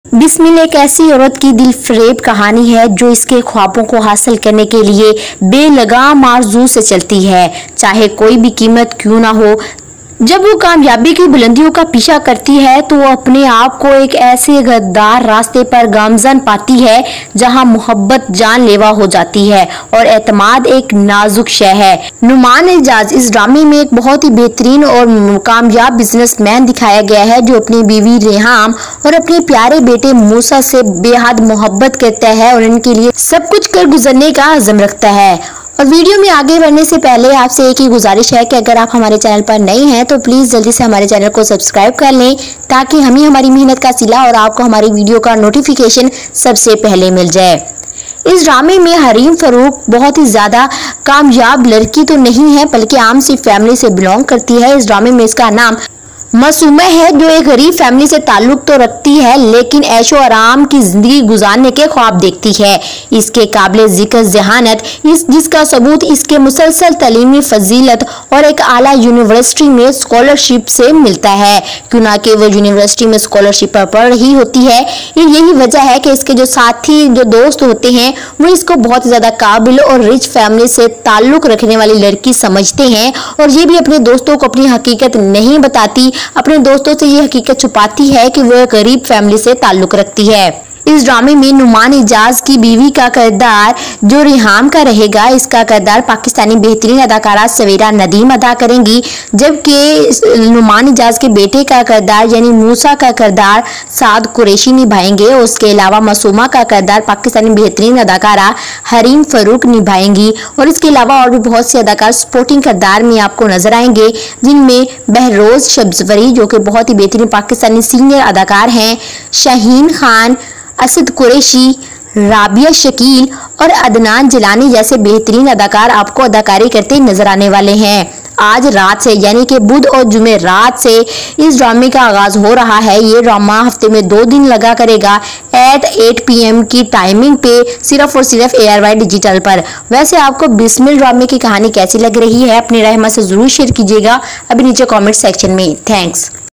Bismil Episode 3 | Naumaan Ijaz | Hareem Farooq | Saad Qureshi | Drama Detail
Assalamualaikum Friends:
In this Vedio I share information about ARY digital new drama BISMIL Complete Story and Cast.   Hareem Farooq and Noman Ijaz  play Lead Role in bismil.

Cast:

Naumaan Ijaz
Hareem Farooq
Savera Nadeem
Saad Qureshi
Behroz Sabzwari
Shaheen Khan
Asad Siddiqui
Rabia Shakeel
Adnan Jeelani

Timing:Bismil every Wed & Thu at 8:00 pm,